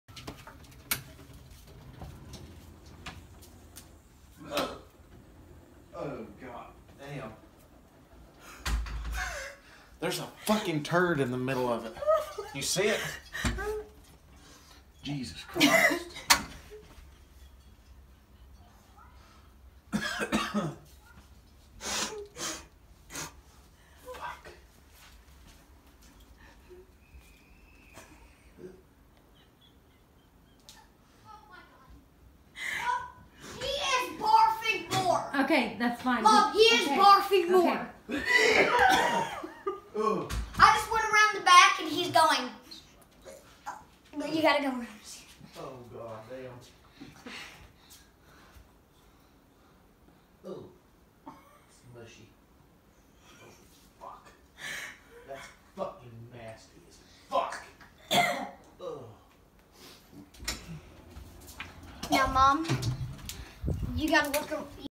Dog eats shit and then vomits it back up on the floor.
Dog eats shit and then vomit sit back up on the floor.